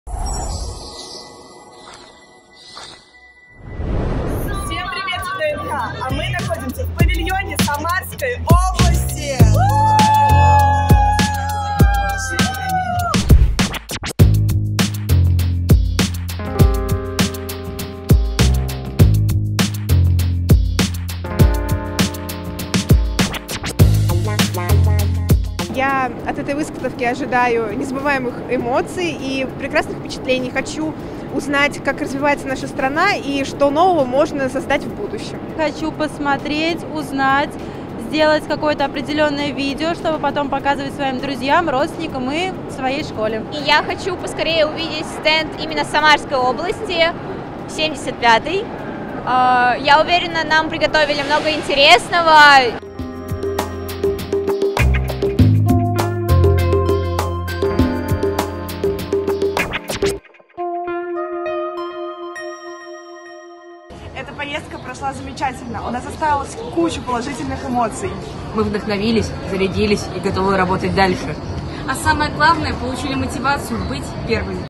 Тольяттинские школьники побывали на выставке-форуме "Россия"
Первая молодежная делегация из Самарской области – всего более 100 человек – побывала на ВДНХ. В столицу отправились ребята, добившиеся успехов в различных областях, представители творческих коллективов, победители и призеры соревнований, юные ученые, активисты реготделения «Движения первых», призеры окружного этапа Всероссийской олимпиады школьников, волонтеры.